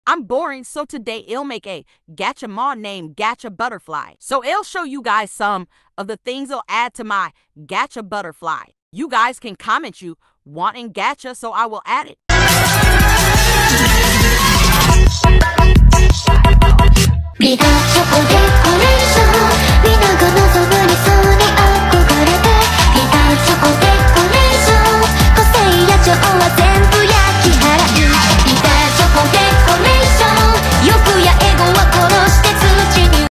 things ill add to Gacha Butterfly! (still working it) u can comment what should i add next!
lemmie talk 'bout this Gacha Butterfly a little bit
i still work on it and idk if i should upload it or not cause im not good at drawing much and afraid ill got frames and im afraid that no one will install it ☠️🎀
so yeah